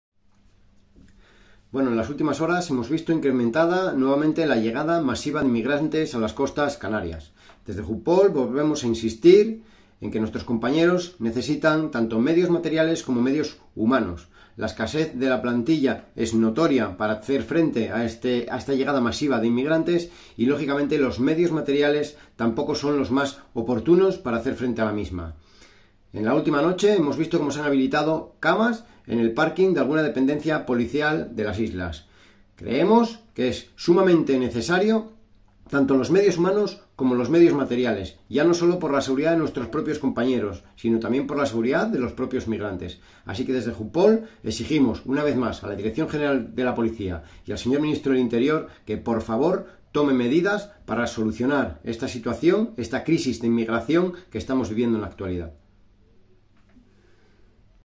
Pablo Pérez, portavoz de JUPOL pide refuerzos para controlar la inmigración en Gran Canaria
Pablo Pérez, portavoz de JUPOL pide refuerzos para controlar la inmigración en GranCanaria